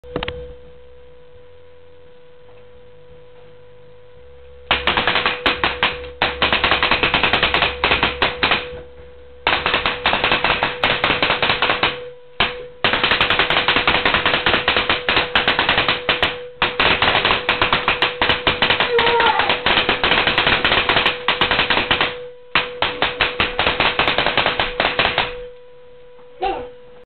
Spyder Pilot ACS 05' Noid Clicking
i just got my new powerex 9.6v charger n battery and im just posting a vid of my noid clicks lol. the gun is all stock.